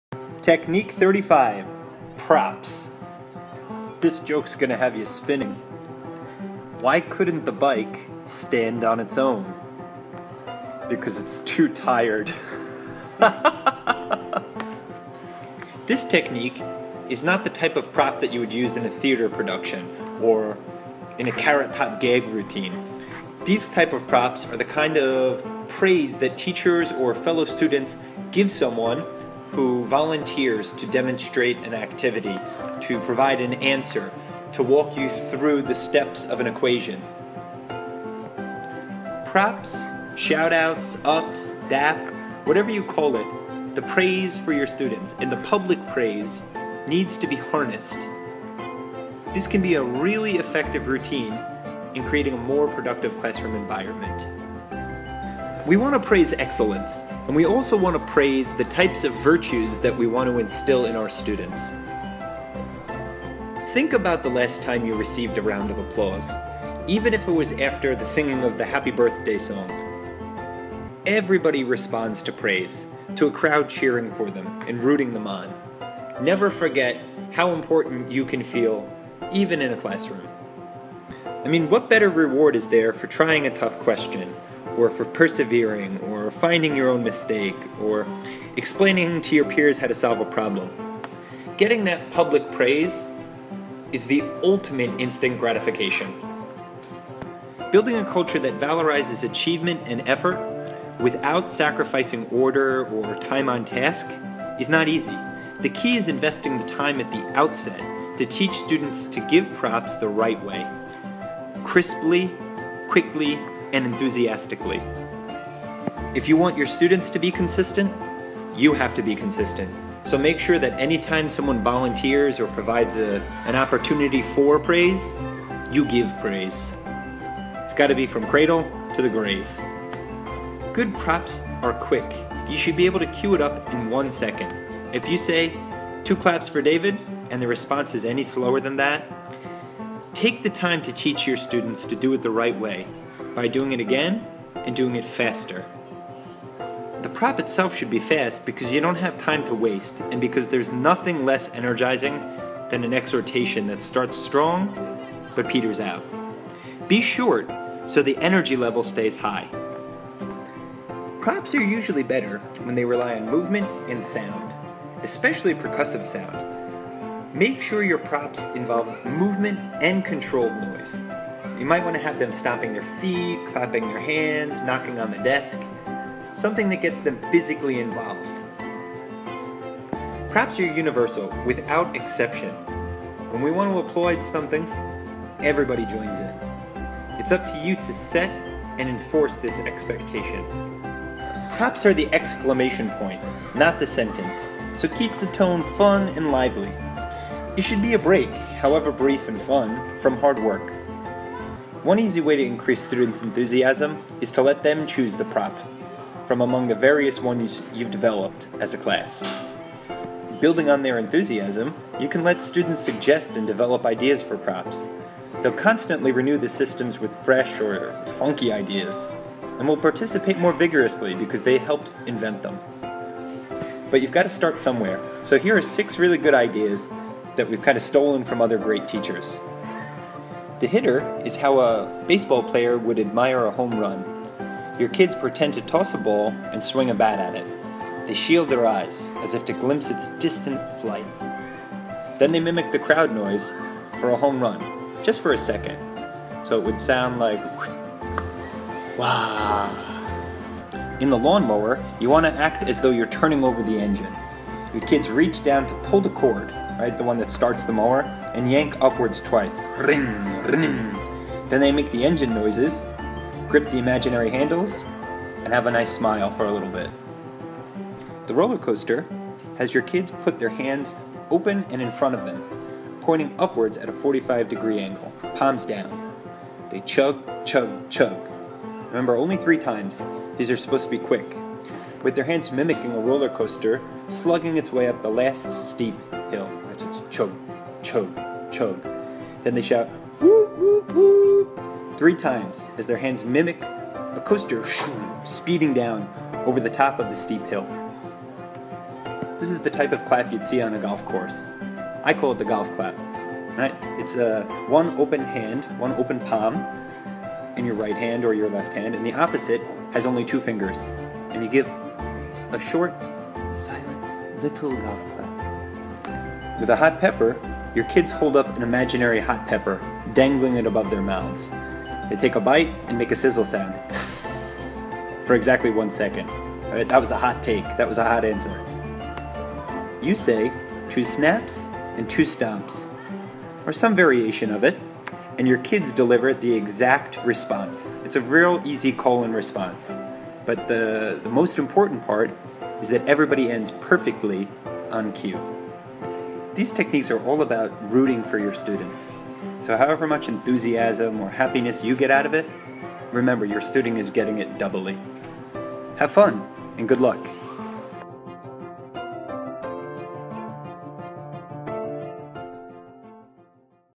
Teach Like a Champion Technique 35 - Props
Learn just how important public praise can be to creating a positive classroom culture.